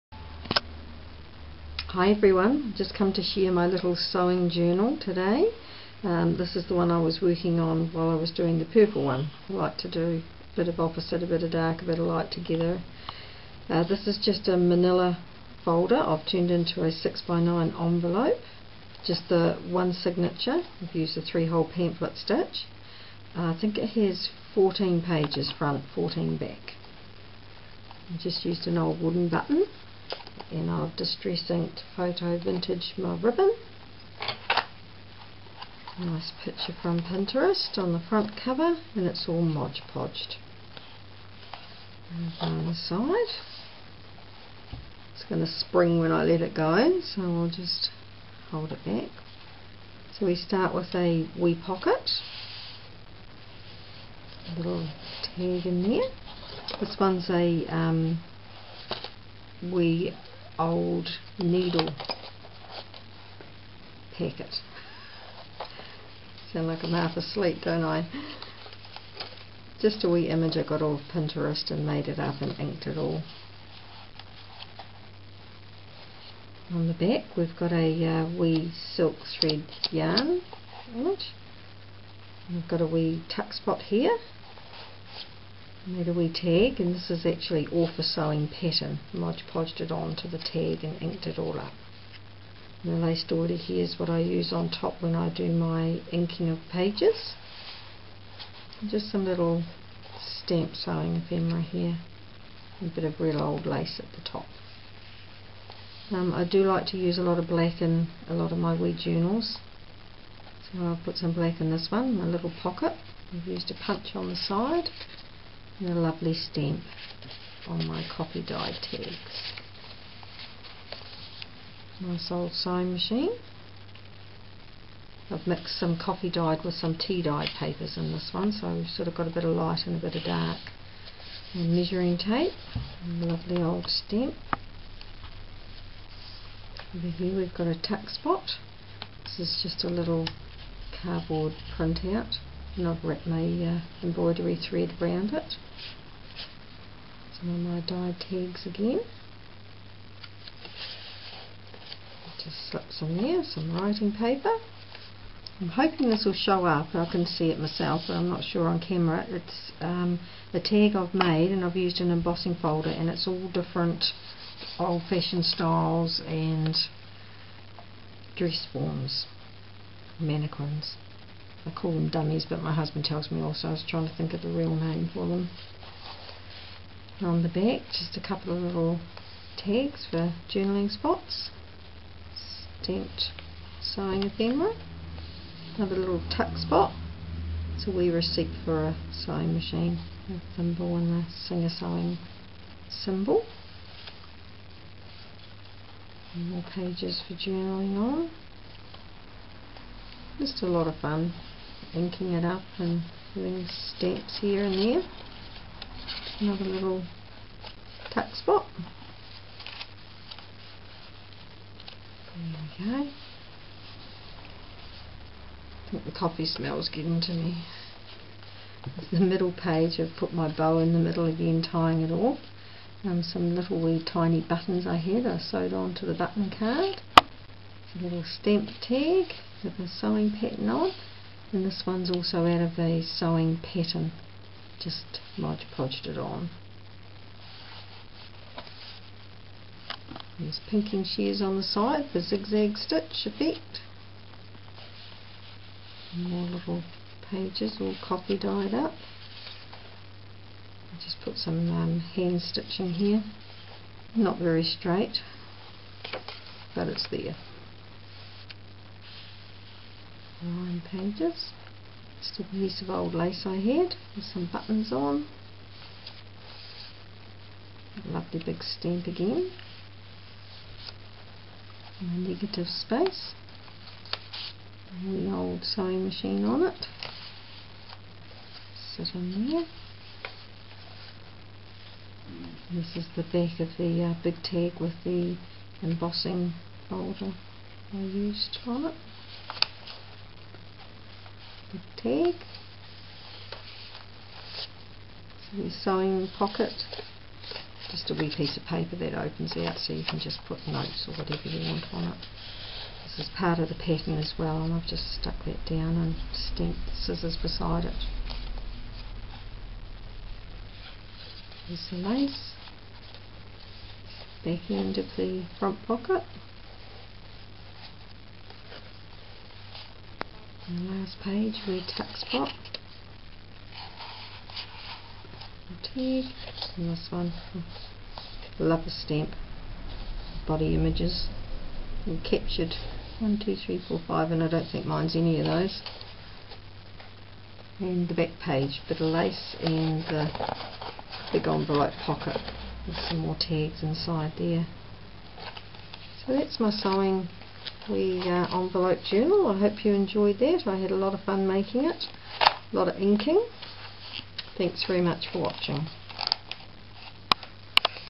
Sewing Envelope Journal
I used a Manilla Folder to make this 6x9in Envelope Journal
 (It has 28 front pages and 28 backs not 14 as I said in the  video sorry)